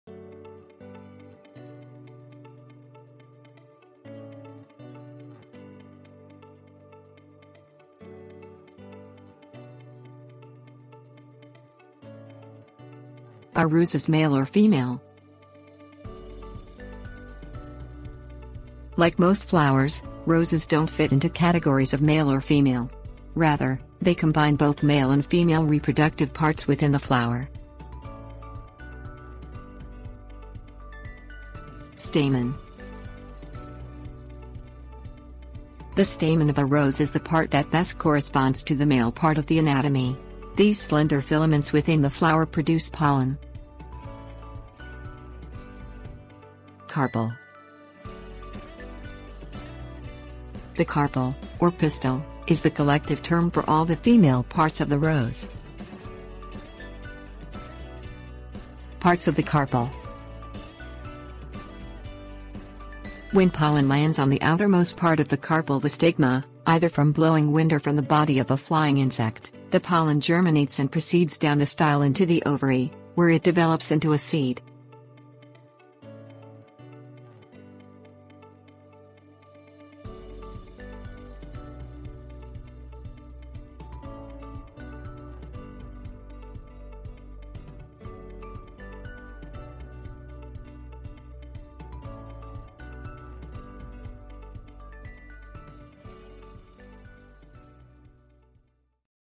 Are Roses Male or Female?
Are Roses Male or Female?. Like most flowers, roses don't fit into categories of male or female. Rather, they combine both male and female reproductive parts within the flower.

Table of contents Are Roses Male or Female?
Stamen 00:30
Carpal 00:45
Parts of the Carpal 00:57